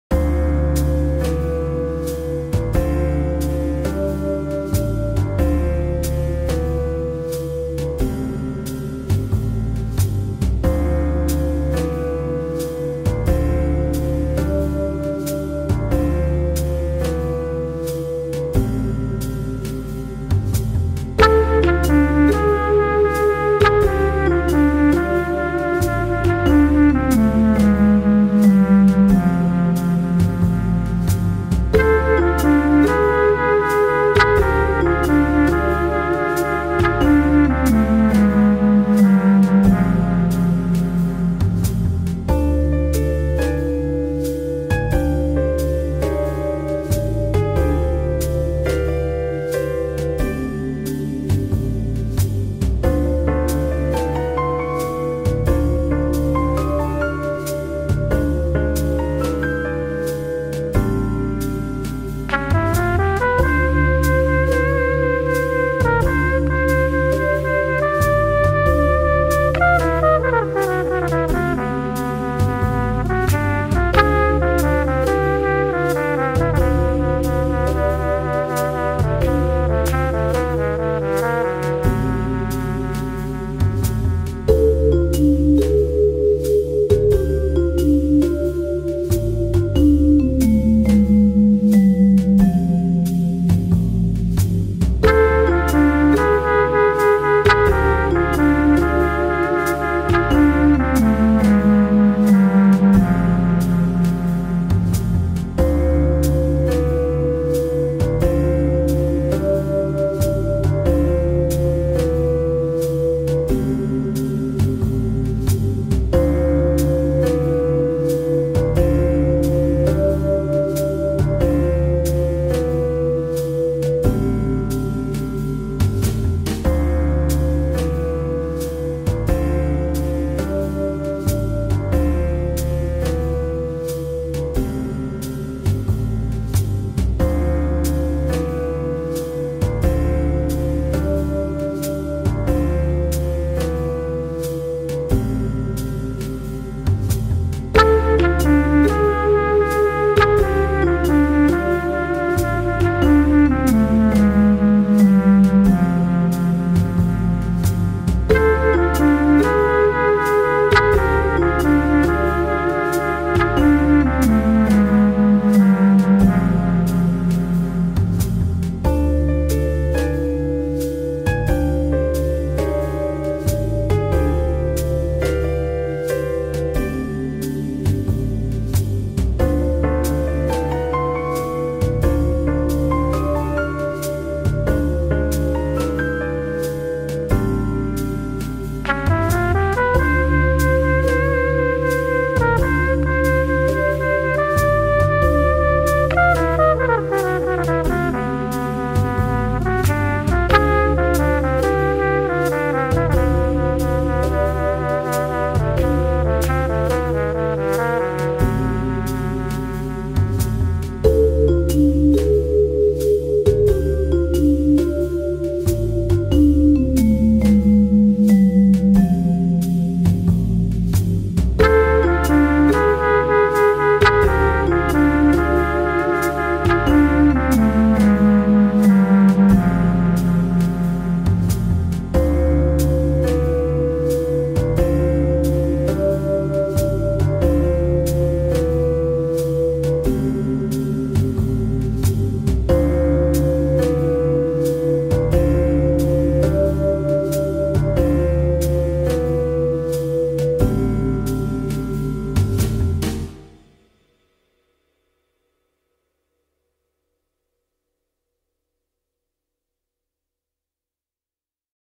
Jazzy Hearts Sensory Motion Background Music Videos
Sound effects and music  combined with motion backgrounds